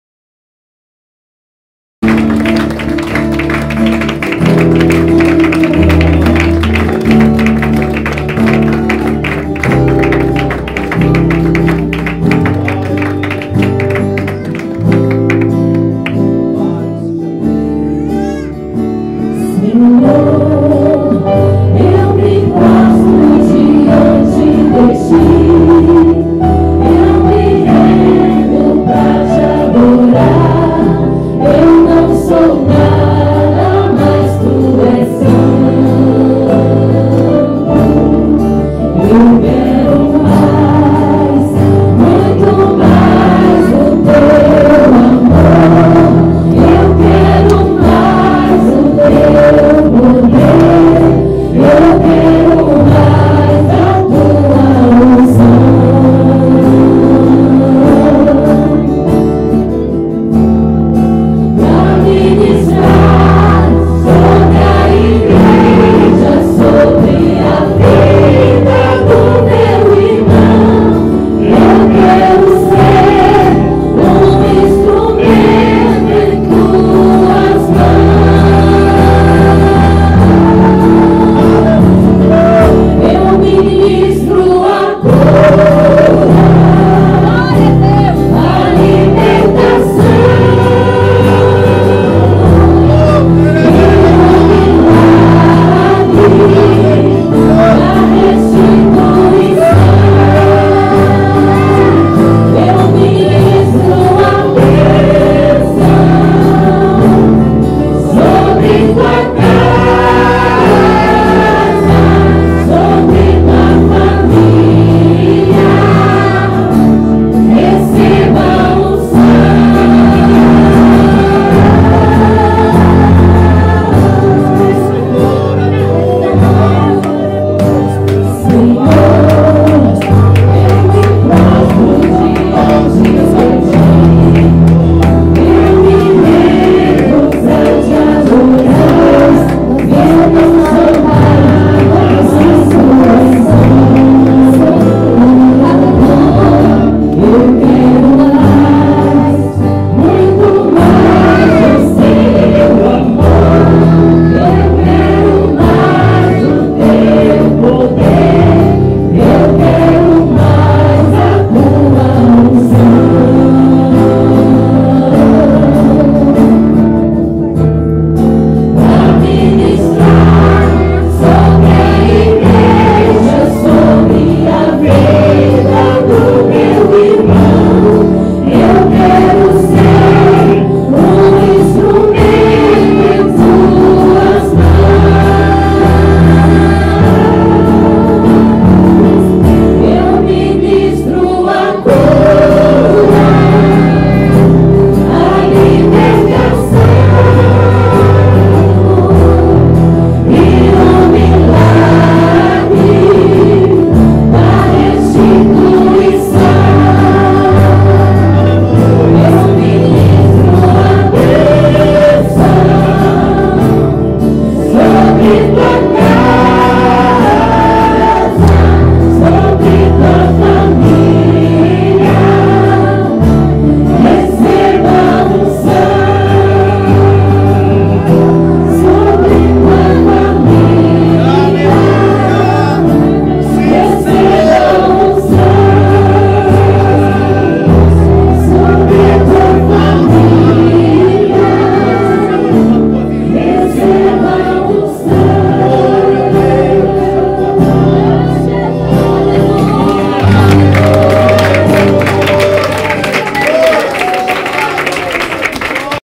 Slides de Fotos da Visita do Pr Roberval 18-3-2012.wmv
Estamos em uma campanha em que os cultos de sabado, estara' sendo realizada uma visita de um pastor de outra congregação, e no dia 17 de Março, esteve em Komatsu, o Pastor Roberval Damno e a Missionaria Andreia Damno, de Yokkaichi. Um grande exemplo e firmamento de fe', foi passado, juntamente com o firmamento de unidade e dedicação, ministrada pela missionaria. Gloria a Deus ! Pela rica oportunidade, abençoada pelo Pastor Takayama e Missionaria Cida.